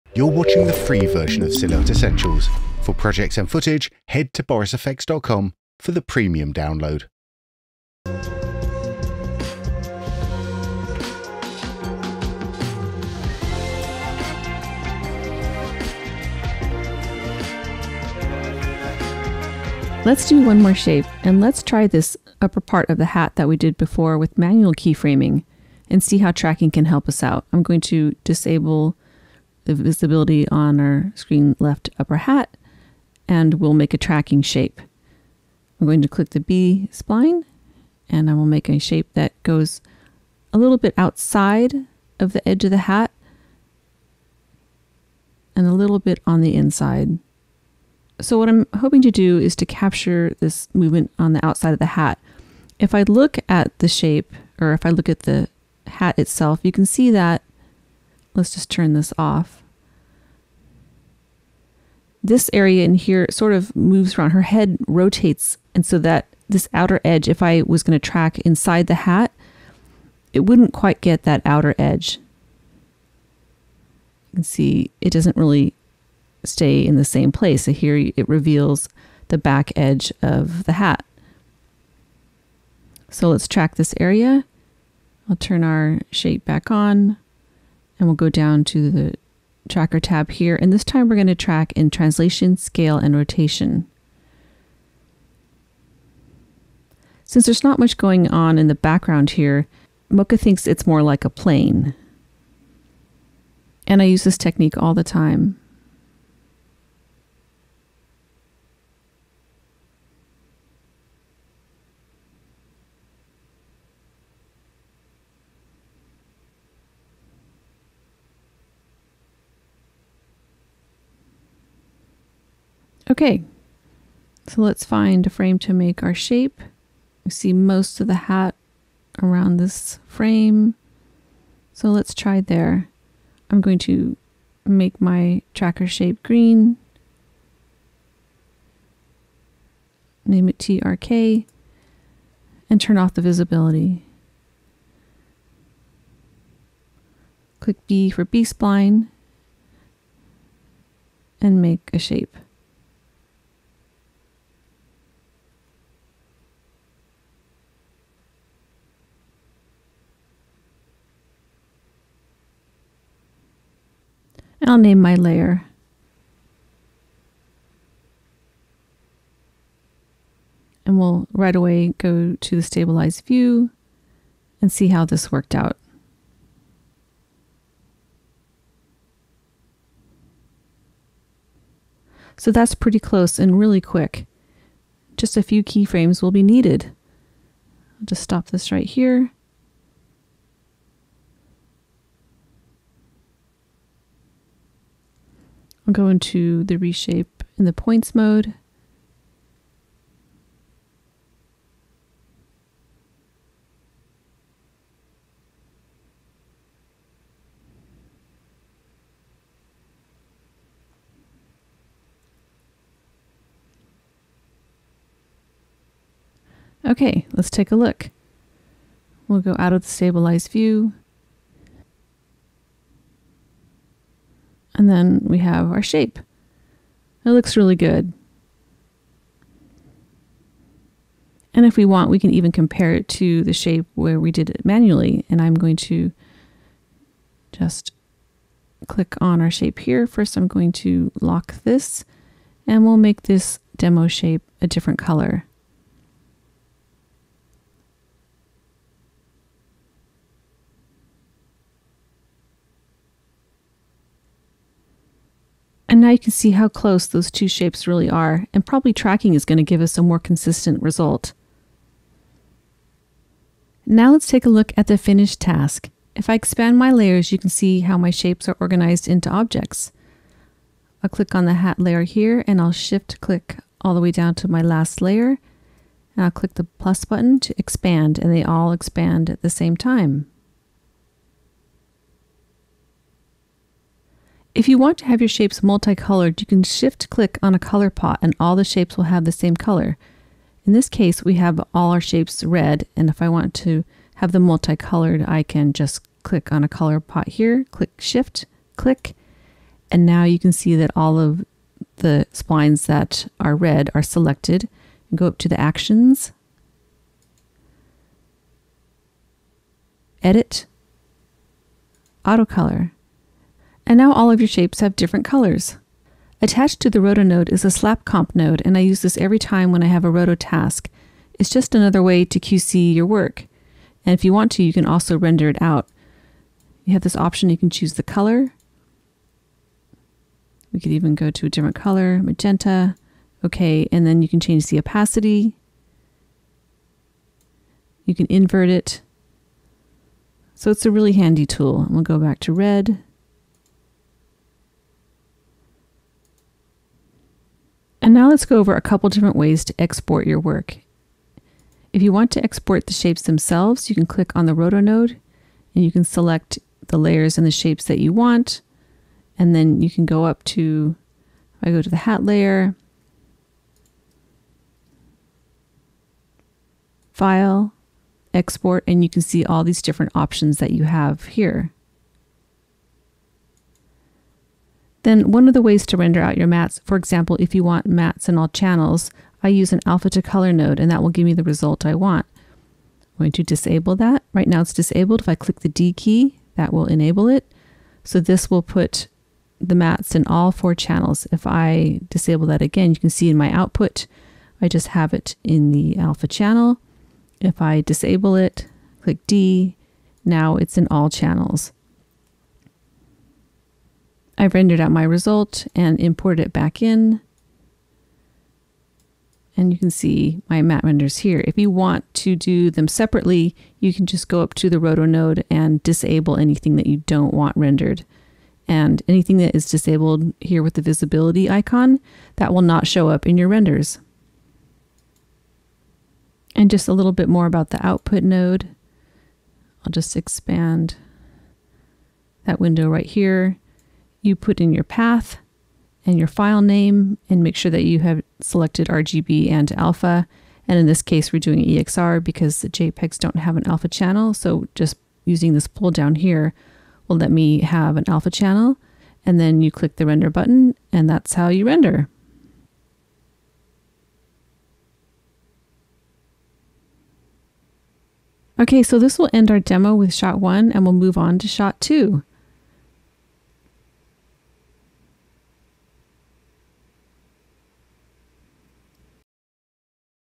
Silhouette Essentials Roto - 01I Hard Edge Finishing [Boris FX rotoscoping]
This is the free version of Silhouette Essentials - Roto with Katie Morris, digital VFX artist and trainer.  In the final section for this shot, we continue the focus on motion tracking with the Mocha Planar Tracker.  We compare the results between manual tracking and the planar tracked shape, and discuss which one gives the better, faster result.

We then look at the finished project and see how to organize, color and export these from Silhouette into other software. This is also the first look we get at the Output node in this course.

For a downloadable version of the full training course, including projects and media, for the Premium series.

Chapter List
00:11 Planar Tracking
04:15 The Finished Project
04:35 Recoloring all shapes
05:39 Exporting Shapes from Silhouette
06:57 Quick Look at the Output Node

Footage is provided courtesy of FASTFOKUS ) Special thanks to Damien Vergez for his contributions.